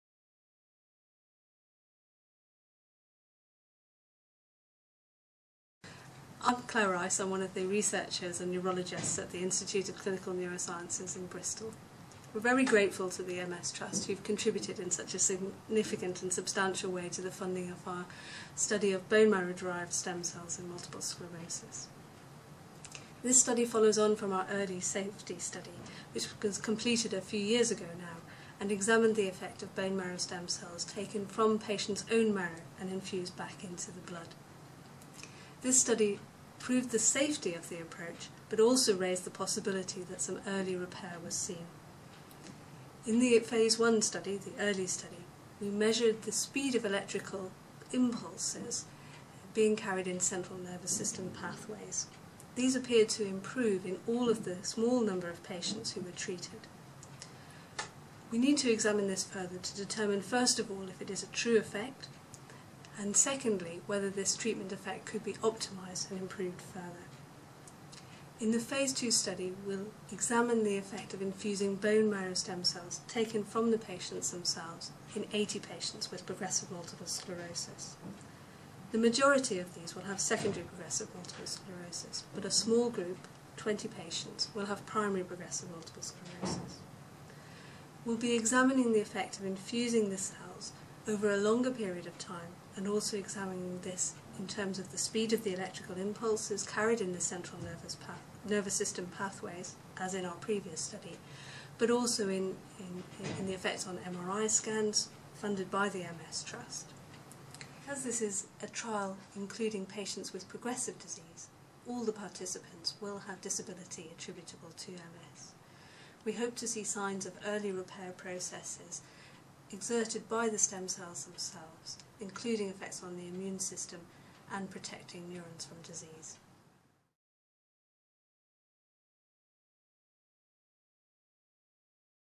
MS Trust Research: funding pioneering stem cell research in Bristol
Help improve the lives of people with progressive MS

The MS Trust has recently invested in research to find a successful treatment for progressive MS. In this video Claire Rice, Lead Researcher, gives more information about the bone marrow stem cell study. We believe this offers real hope for the future, but we also understand that people living with progressive MS now need the highest levels of care and support to ensure that they have the best quality of life possible.